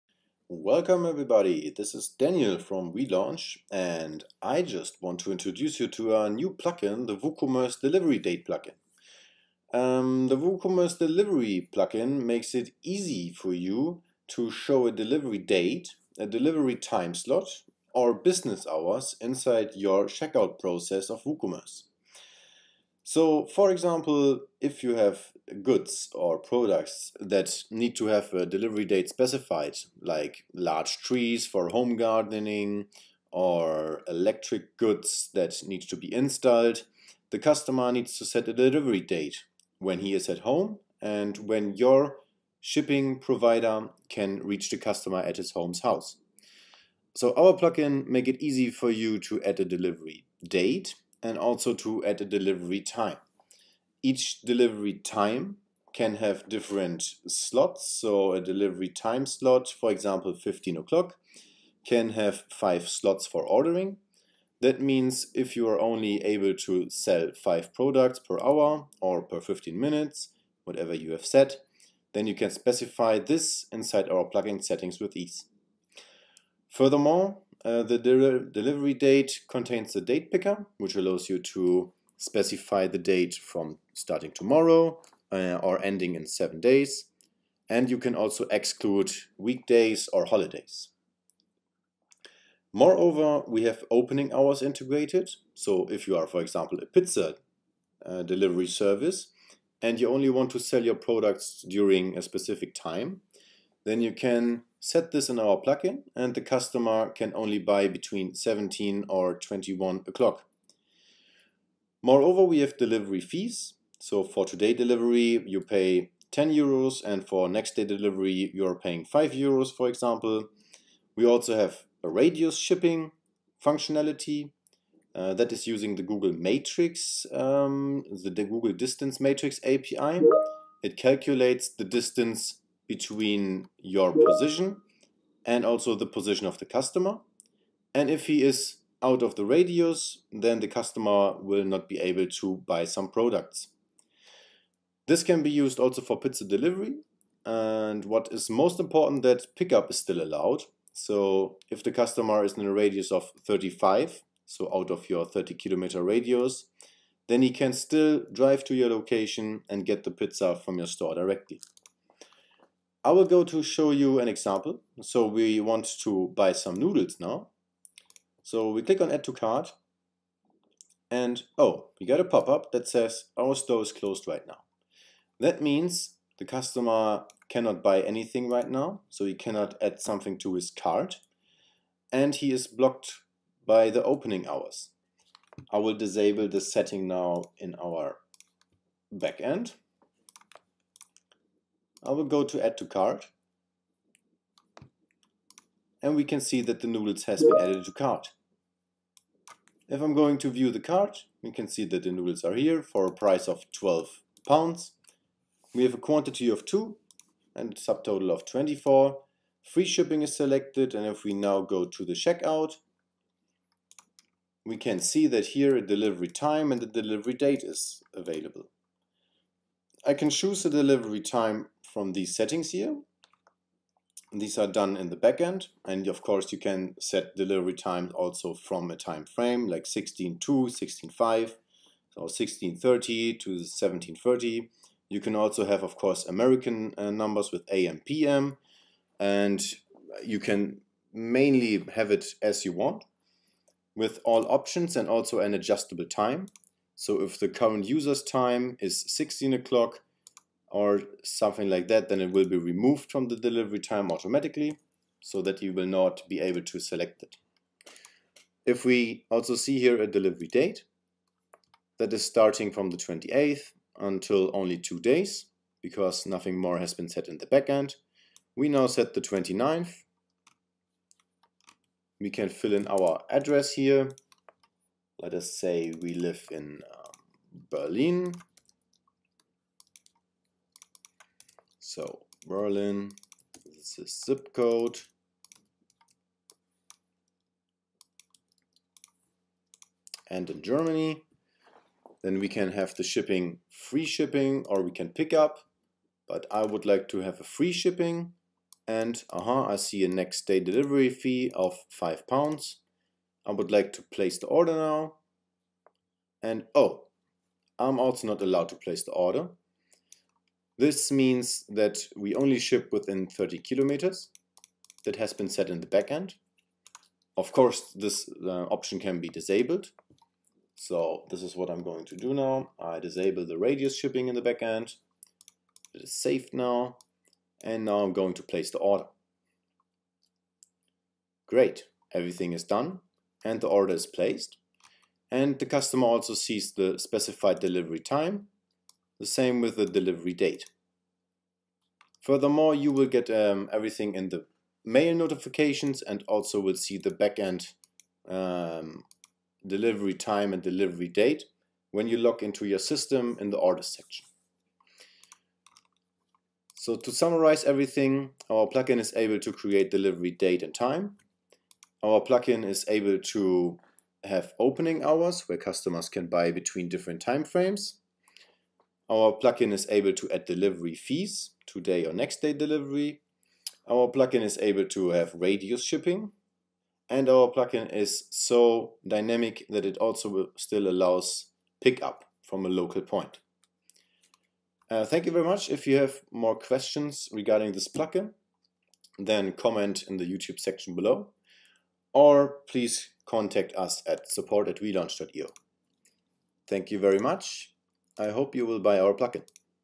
WooCommerce Delivery Plugin
Show delivery date, delivery time slots or opening hours during the WooCommerce checkout process.